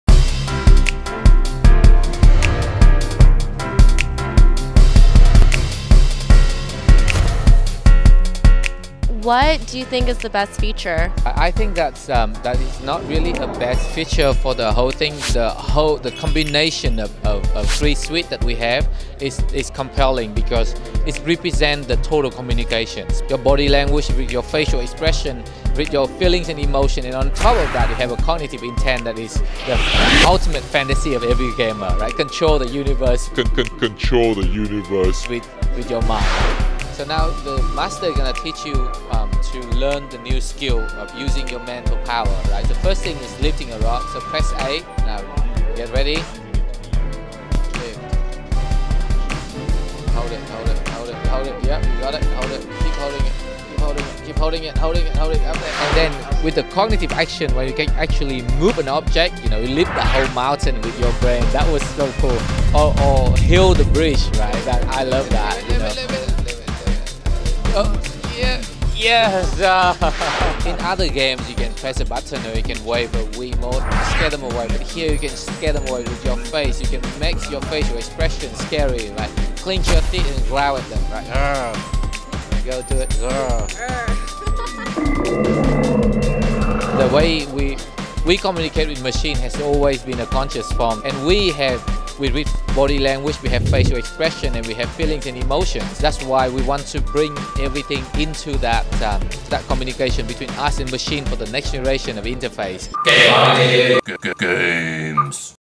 Emotiv Interview
This is another video I edited for GameZombie.tv.  This is an interview with Nan Do, CEO of Emotiv.